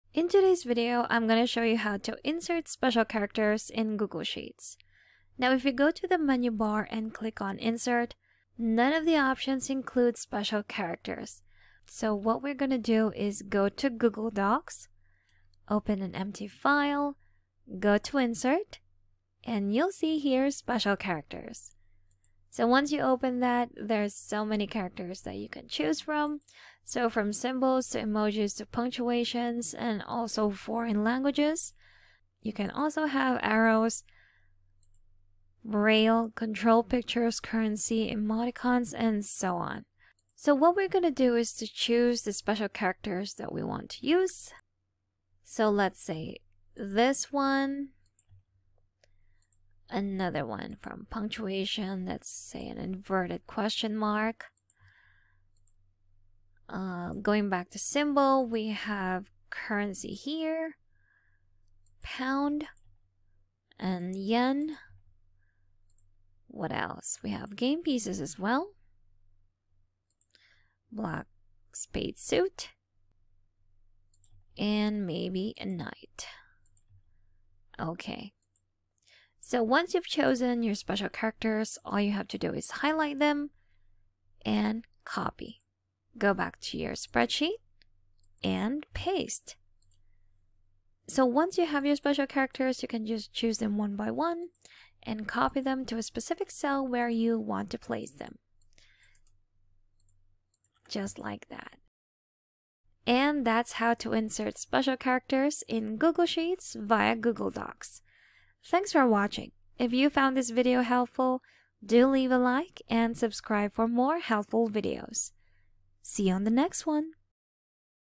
How to Insert Special Characters in Google Sheets (with Google Docs)
In this video, learn how to insert special characters in Google Sheets!

I'll guide you through a simple yet powerful tutorial on enhancing your spreadsheets with symbols and unique characters. Uncover valuable tips and tricks to efficiently communicate data and make your sheets visually appealing.